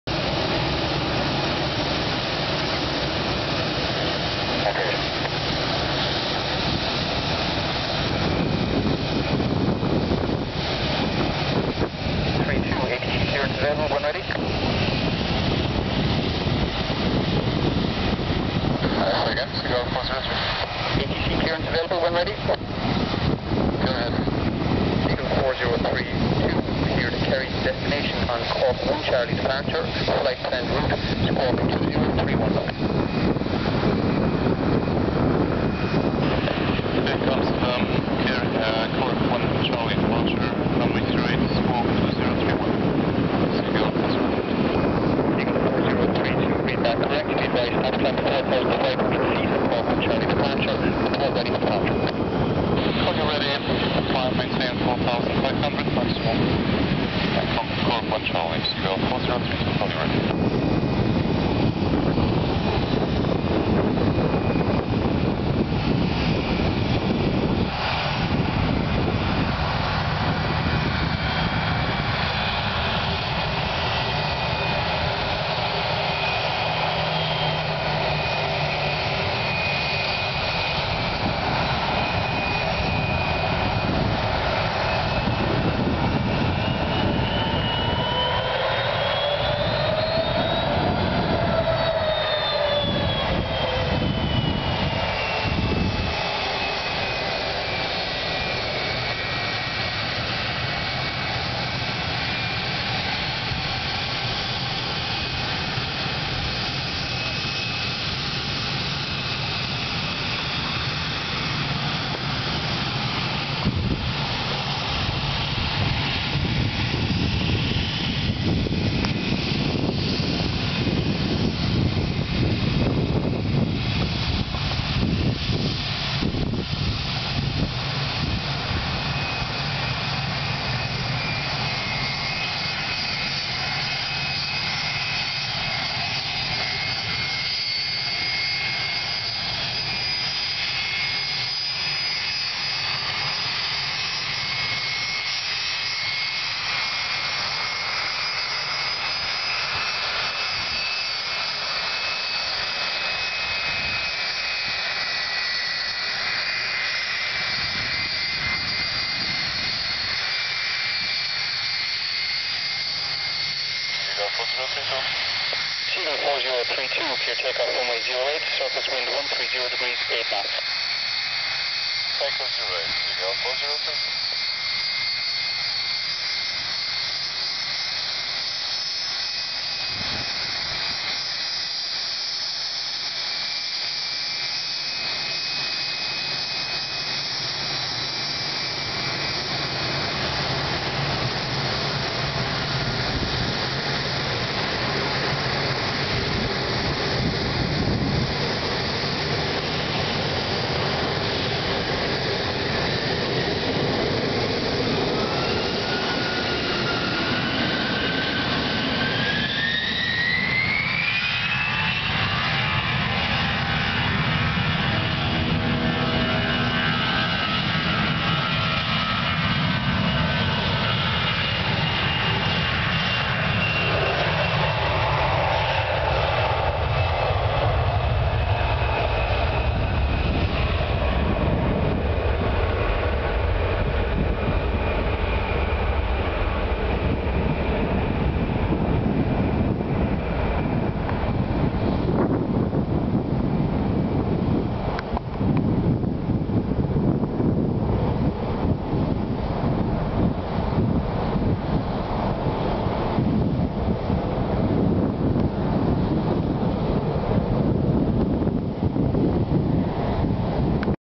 Seagle Air Airbus A320 departing Kerry Airport
A Seagle Air Airbus A320 is taxiing to runway 08 for departure on 21st October 2009. At the same time a Thomson/First Choice Boeing 757-200 has to hold on runway 26 and wait for a parking stand on the crowded apron. Because of a big google convention in nearby Killarney the rather small Kerry Airport (KIR/EIKY) today saw an unprecedented amount of traffic and more plane types and airlines than usual (B737, B757, A320, MD90 and Challenger 300 from Hello, Seagle Air, Thomson/First Choice, Ryanair and Air Berlin).
You may notice that no destination is mentioned in the radio communication, and that was the same with all google-planes departing that day. It seems to have been a somewhat secret operation...